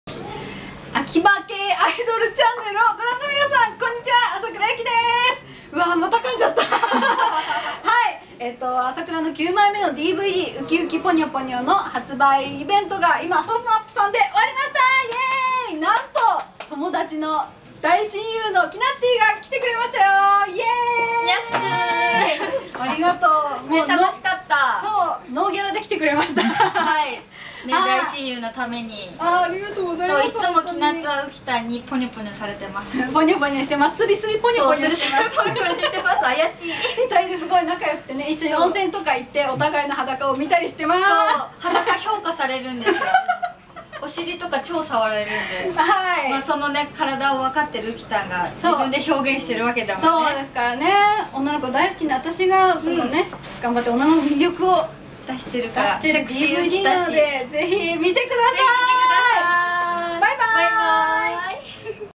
浅倉結希動画コメント
DVD『うきうきぽにょぽにょ』発売記念イベント。
DVDの全体的な仕上がりは、タイトルに「ぽにょぽにょ」と入っていますが、撮影した頃は1番細く、今よりも4kgぐらい痩せていました（笑）
スレンダーでアダルティーな私を見て欲しいです。
1番の見どころはセクシーなお風呂のシーンですね。
あととにかく景色が綺麗でした。